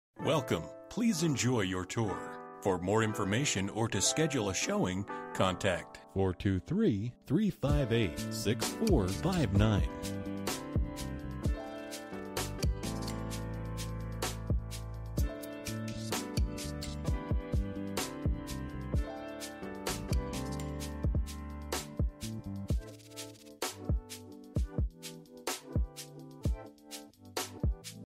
Residential for sale - Tbd Bacon Road, Jonesborough, TN 37659
Lowland wooded lot with a creek flowing through it. lot is partially pasture land and part wooded. This lot is 1.54 acres of parcel id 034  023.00 separated by the road and is currently not surveyed separately. Would be a great purchase along with mls# 9951006

Bedrooms: 0
Bathrooms: 0.00
Square Feet: 0
Price: $15,000